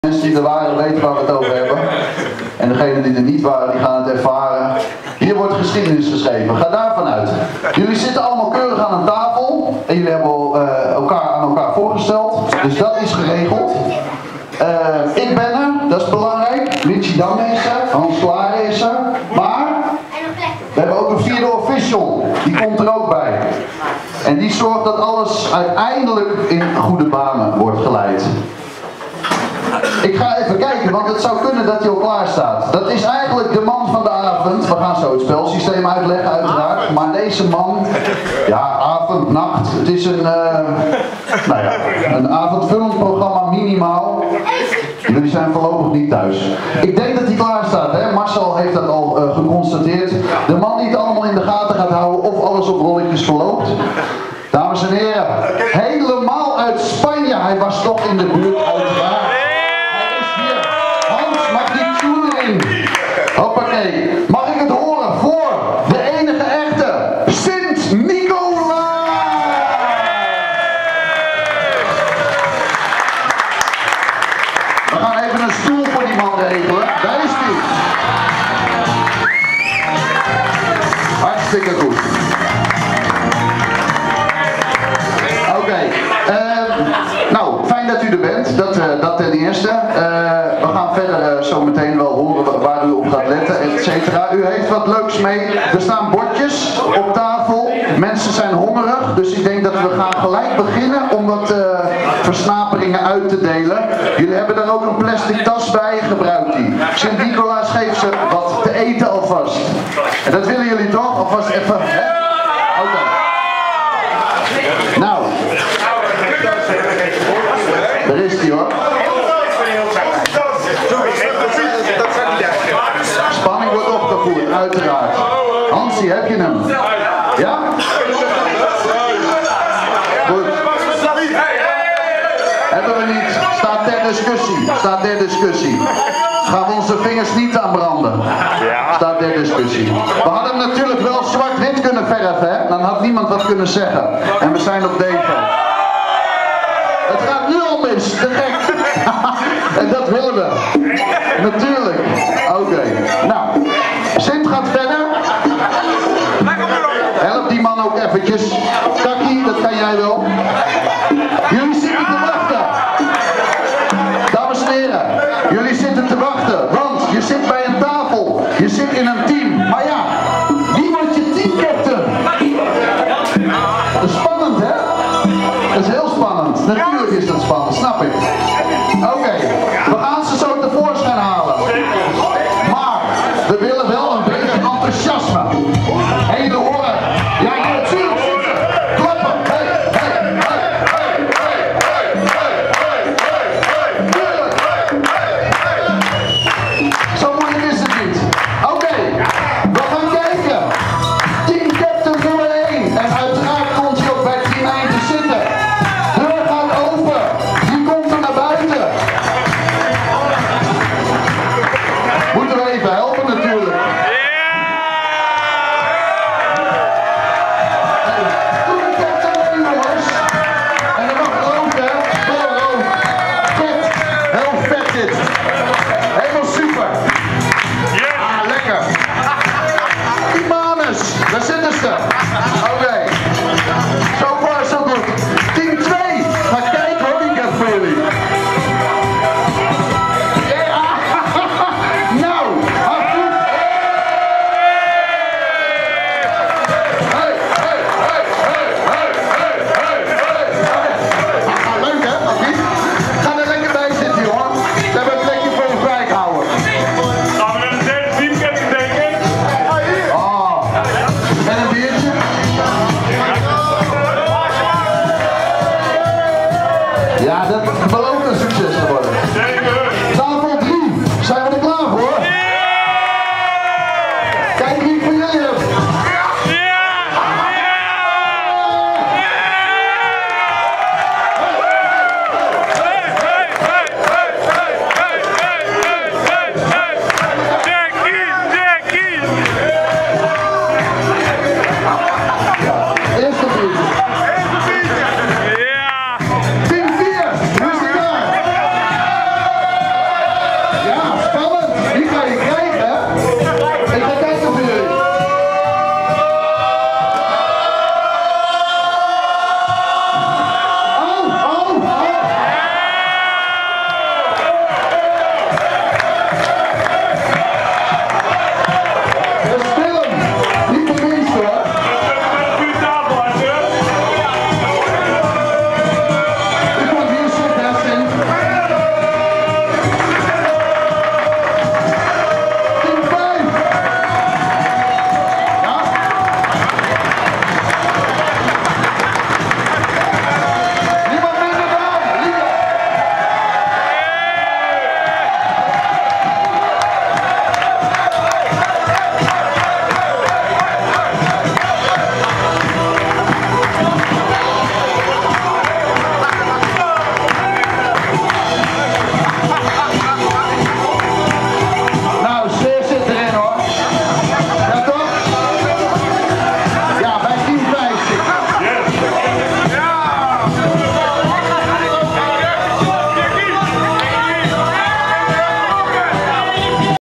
DEVO'58 voetbalquiz 2013: de teamcaptains worden toegewezen
Legendarische Sinterklaas voetbalquiz in de kantine van Devonello, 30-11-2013.